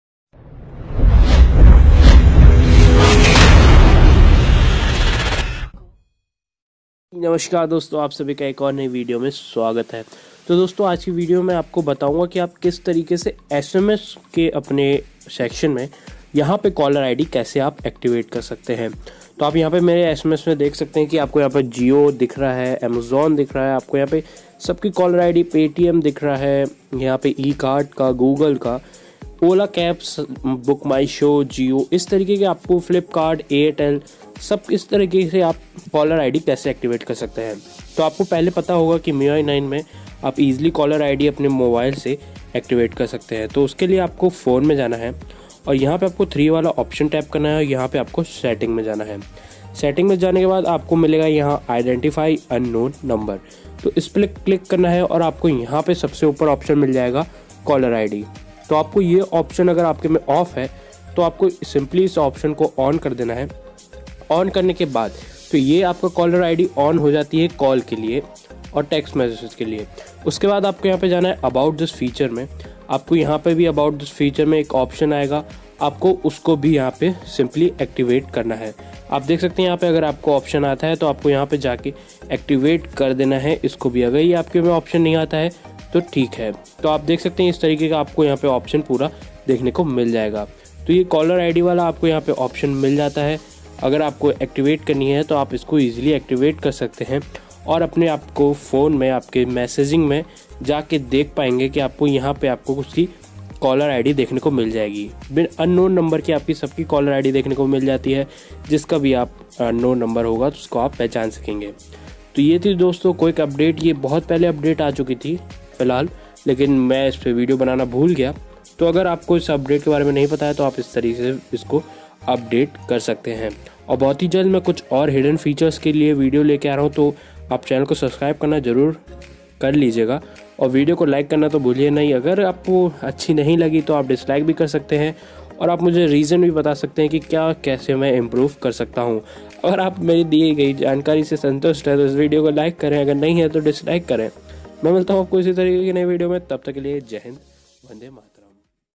How to Activate Caller ID For SMS in MIUI | Latest MIUI Feature Activate Caller ID for SMS
Hello, Friends Welcome to HarshitBhatia!!

Music credit -